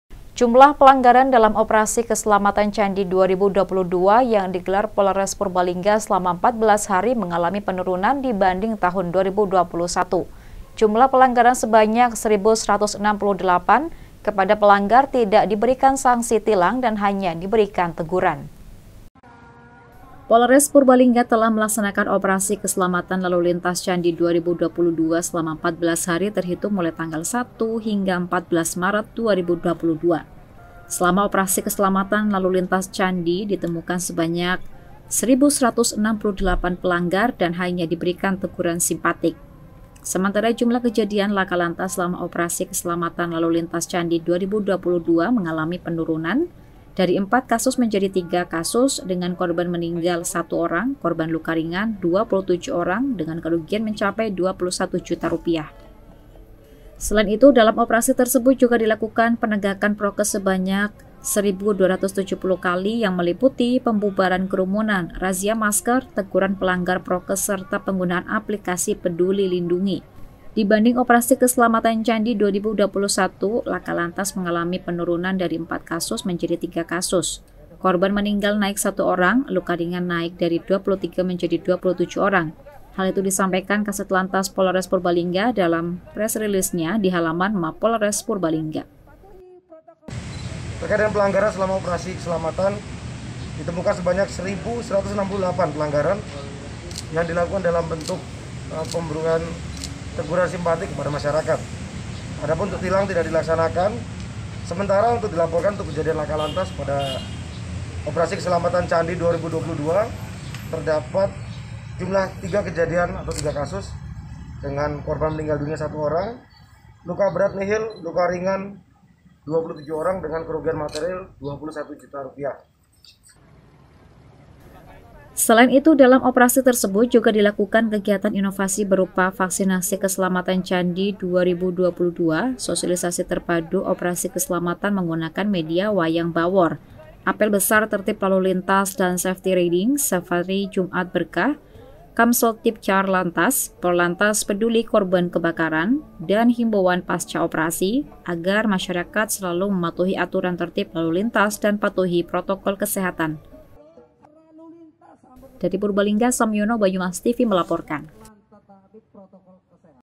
PELANGGARAN DALAM OPERASI KESELAMATAN MENURUN
JUMLAH PELANGGARAN DALAM  OPERASI  KESELEMATAN CANDI 2022, YANG DI GELAR  POLRES PURBALINGGA SELAMA 14 HARI  MENGALAMI PENURUNAN DI BANDING TAHUN 2021, JUMLAH PELANGGARAN SEBANYAK 1168, KEPADA PELANGGAR TIDAK DI BERIKAN SANGSI TILANG DAN HANYA DI BERIKAN TEGURAN.